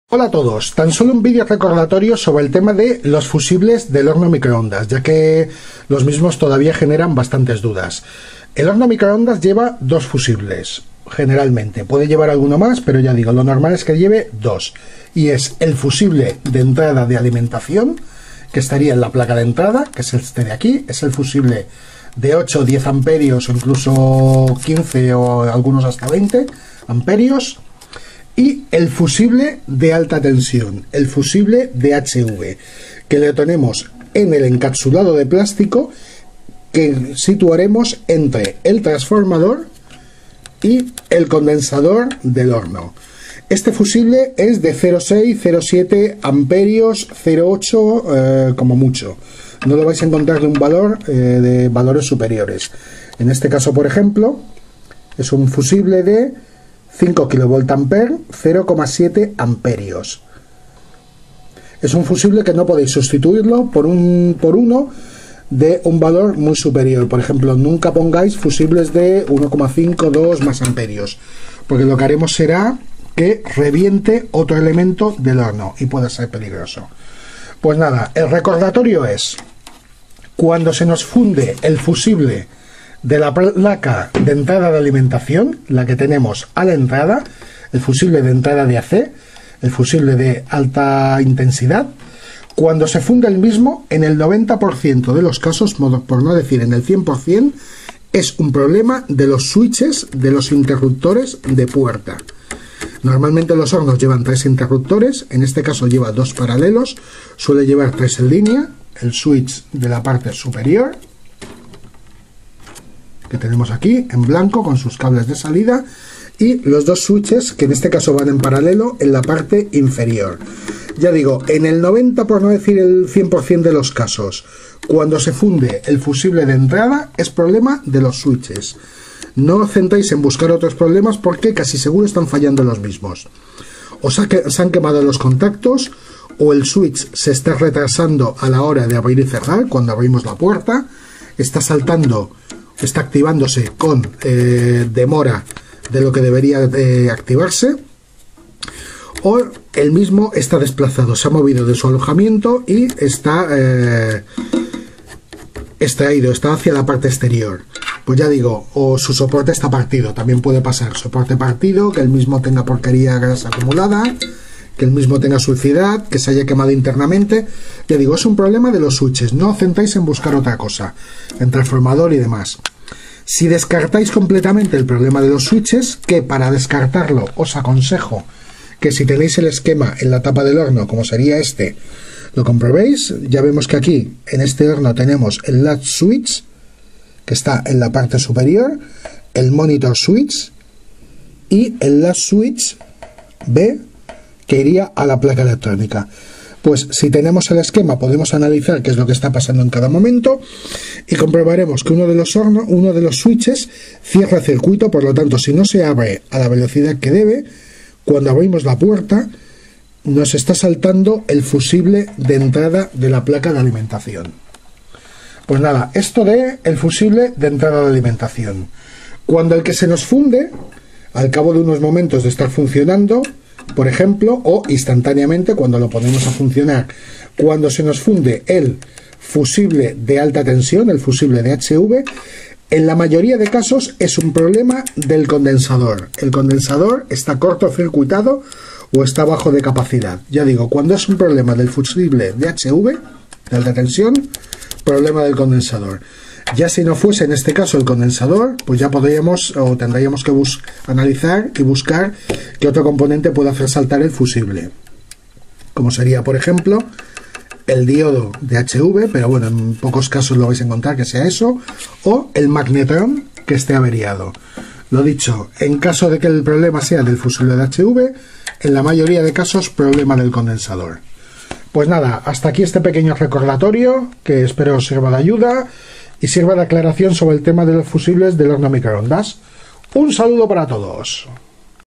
2 FUSIBLES EN HORNOS MICROONDAS ¿PORQUE SE FUNDE CADA UNO DE ELLOS?
La temática de este vídeo, la he tocado ya en vídeos anteriores, pero es un tema sobre el  que me realizan consultas repetidamente, por eso dedico un nuevo vídeo a aclarar porque salta en el 95% de los casos el fusible de entrada de alimentación de  entre 8 y 20A según el horno = Avería de uno de los switches de entrada.
O porque salta el fusible de HV de 0,7A  en el 95% por desgaste del mismo por lo que reponerlo es suficiente, o por  avería del condensador.